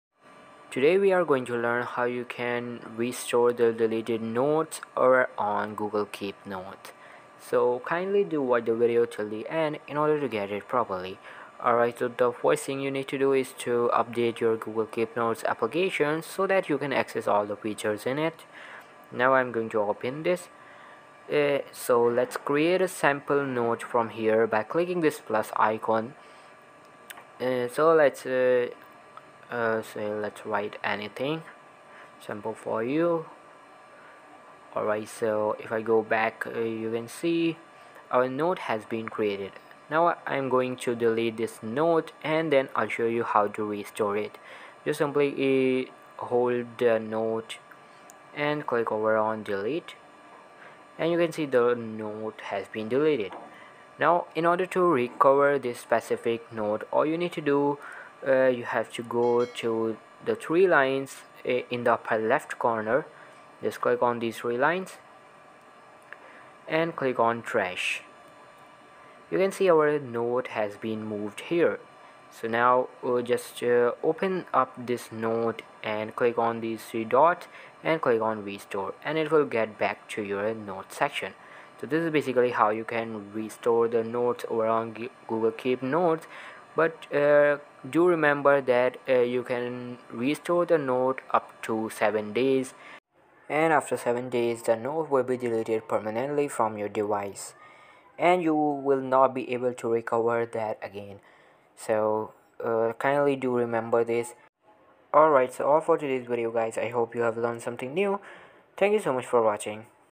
How To Restore Permanently Deleted Notes On Android
In this video we tell you how to restore permanently deleted notes on android. If you deleted some notes for any accident you can get the back easily. In this video we show you how you will get your permanently deleted notes back on your android phone

00:00 Intro
00:05 How To Restore Permanently Deleted Notes